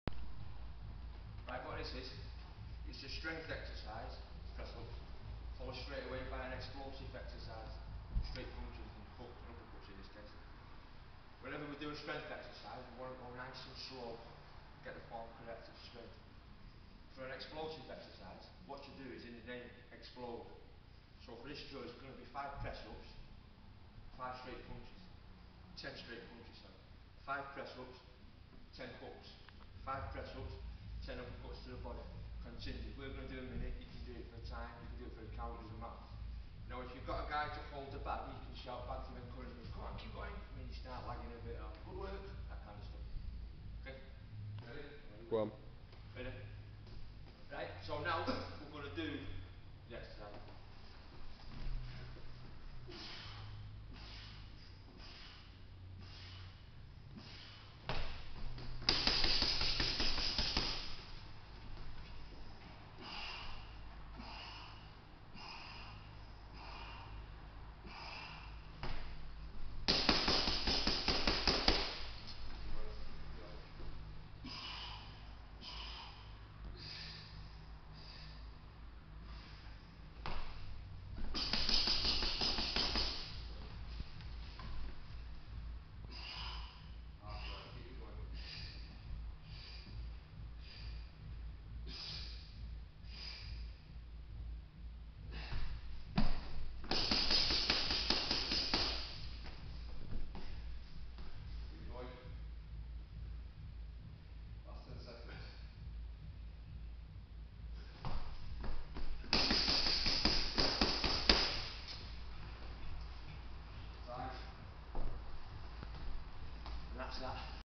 Heavy Bag Drill
Mixing punches on the heavybag along with press ups. For time !